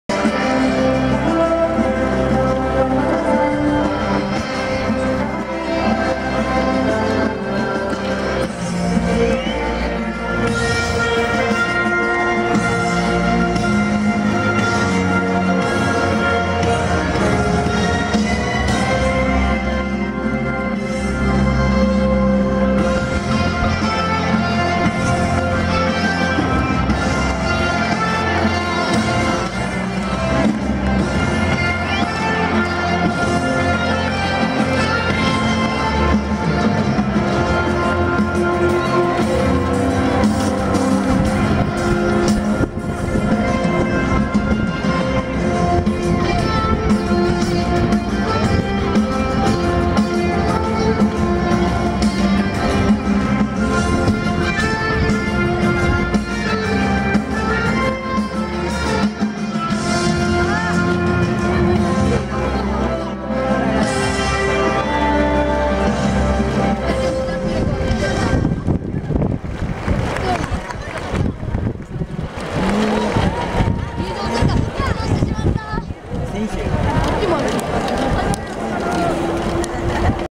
第93系高校サッカー選手権準決勝第1試合選手入場
2015年1月10日埼玉スタジアム2002にて
日大藤沢対星稜